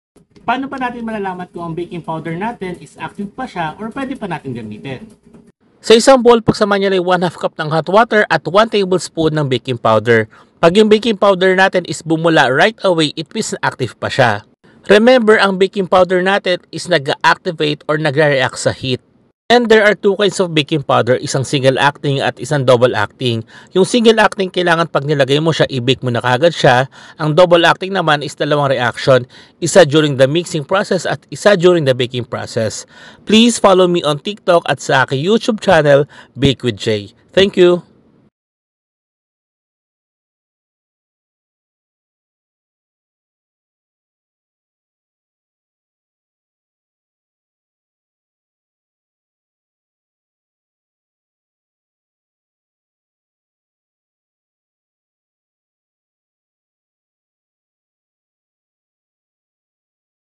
HOW TO TEST BAKING POWDER IF IT IS STILL GOOD TO USE
(chef_jayr)